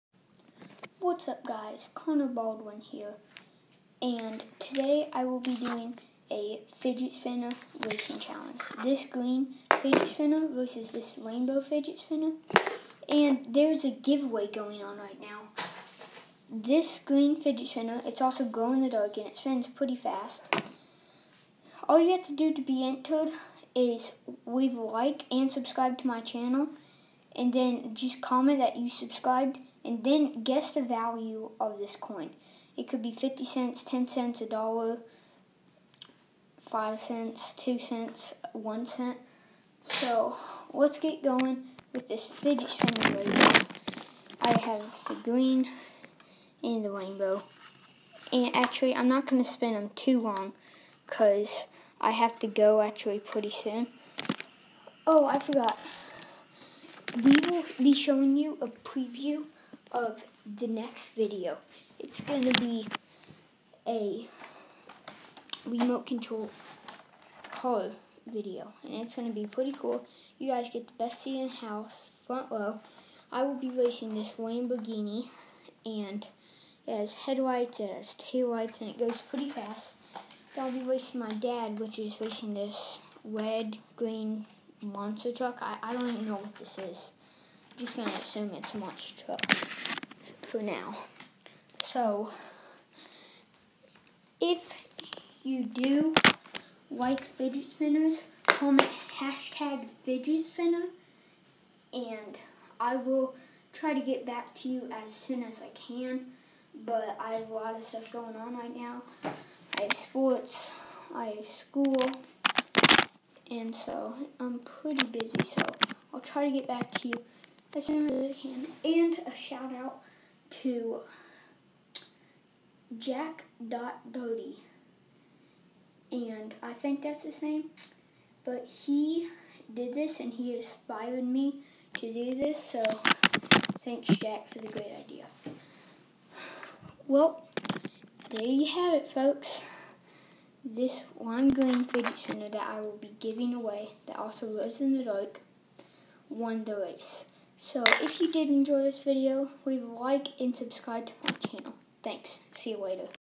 August 3, 2017
1st Time Youtuber Awesome Fidget Spinner Race!